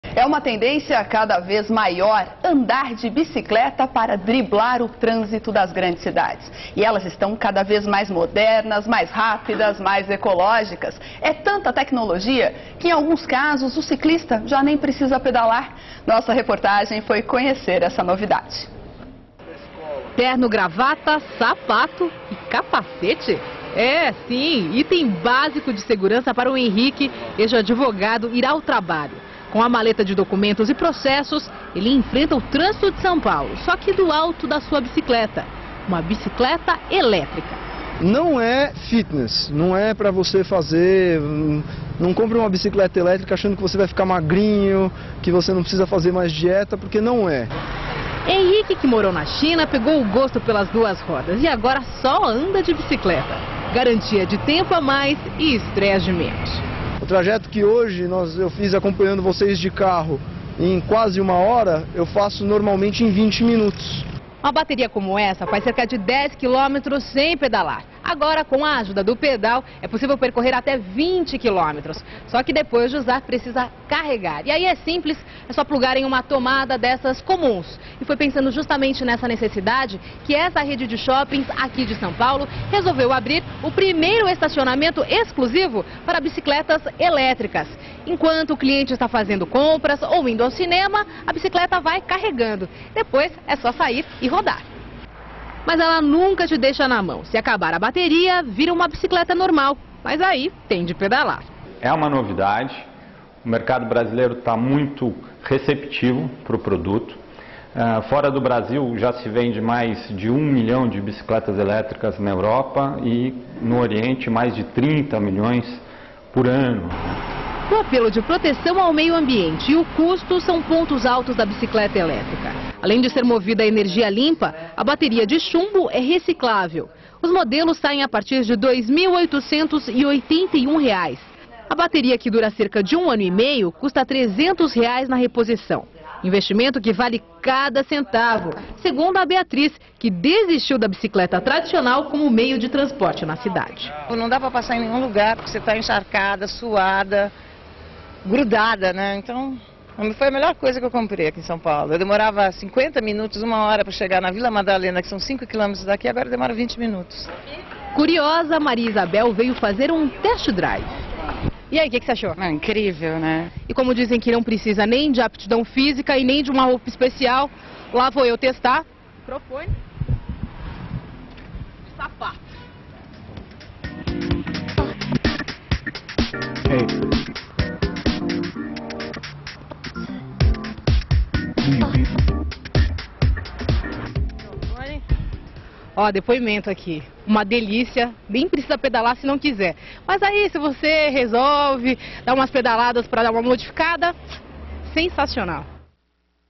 Bicicleta elétrica - 21/04/2011
É uma tendência cada vez maior: andar de bicicleta para driblar o trânsito das grandes cidades. E elas estão cada vez mais modernas, rápidas e ecológicas. É tanta tecnologia que, em alguns casos, o ciclista já nem precisa pedalar! Nossa reportagem foi conhecer essa novidade.

Tema: Transportes